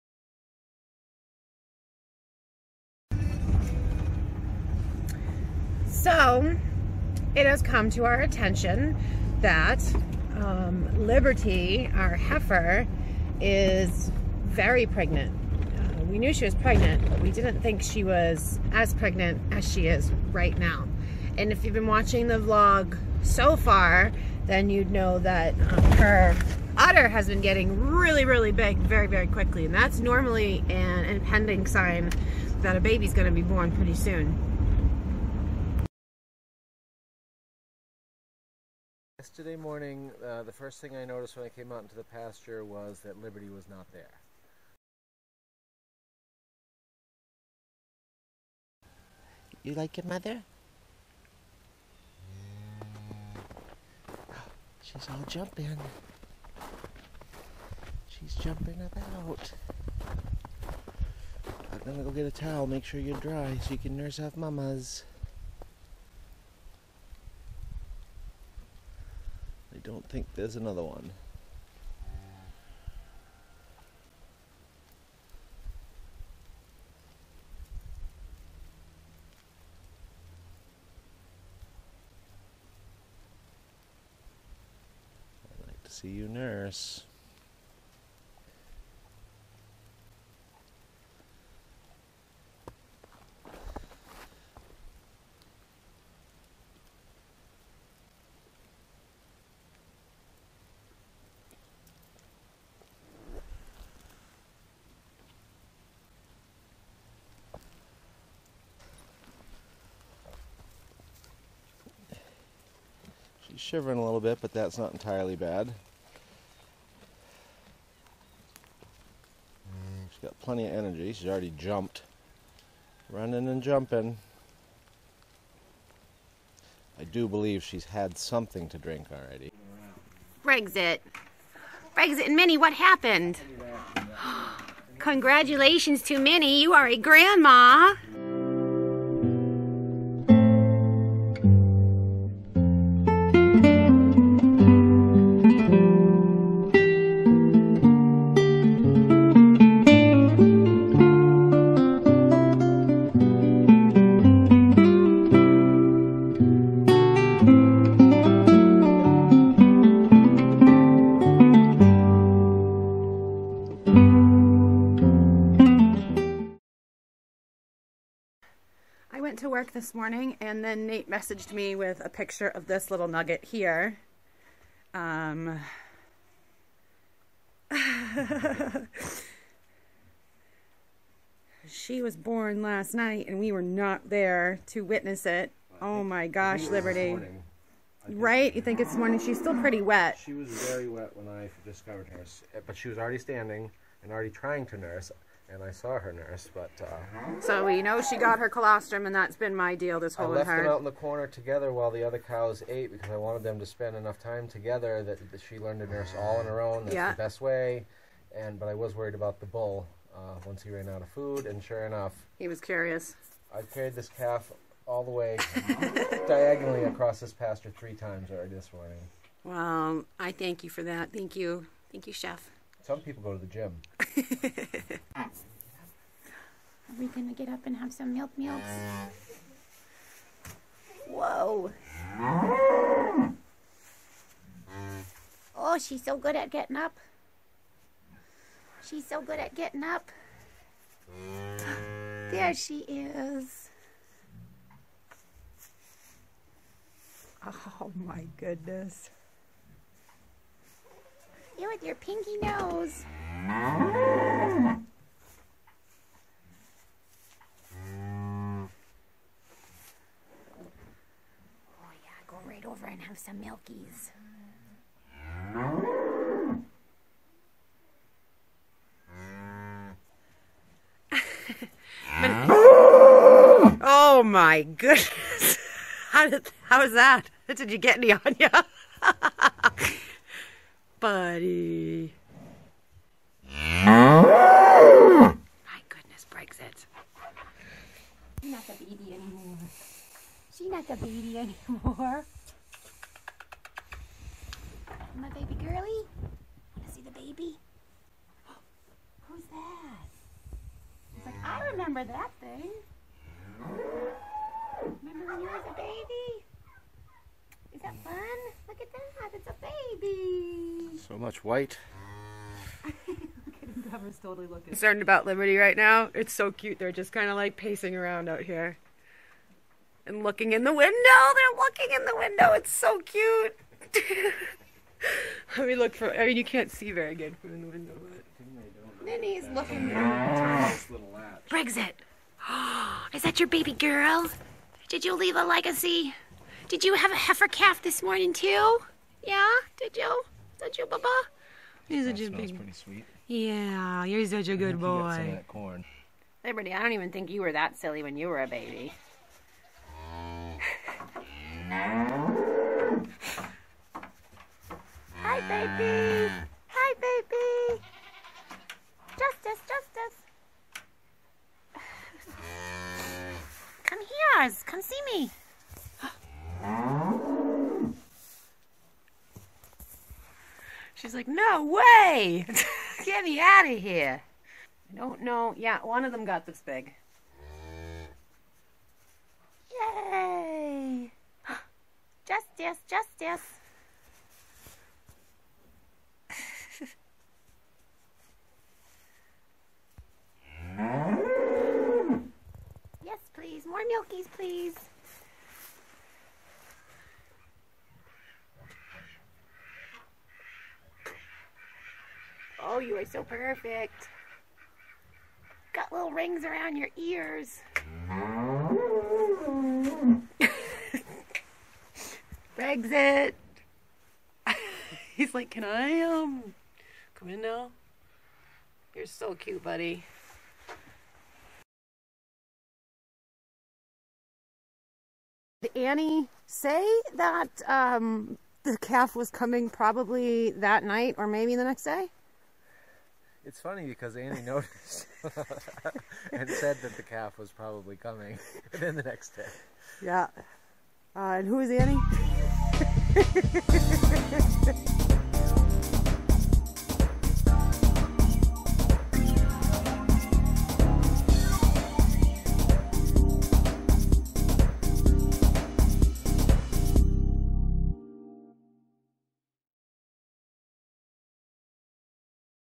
A Calf Born at Bradley Farm
A brief introduction to our new heifer calf, Justice.